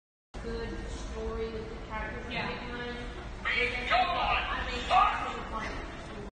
BEGONE THOT!
Taken at Birmingham collectormania 25 on Sunday

Soundclip played through the suits built in speakers

Foxy is me
Video taken by scott